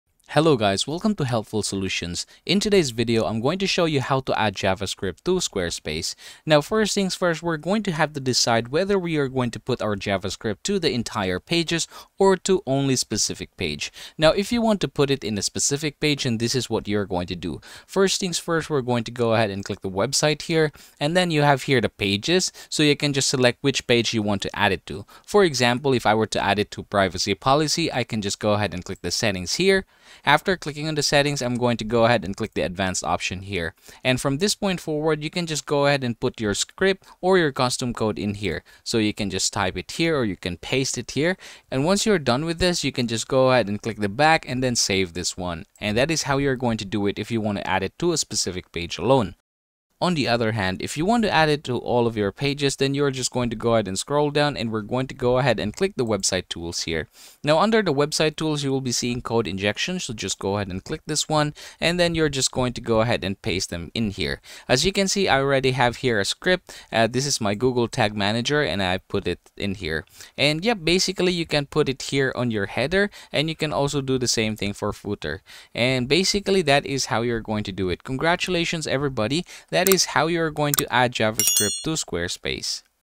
How To Add Javascript To Squarespace (How To Install Custom Javascript On Squarespace)
How To Add Javascript To Squarespace (How To Install Custom Javascript On Squarespace). In this video tutorial I will show you how to add Javascript to Squarespace.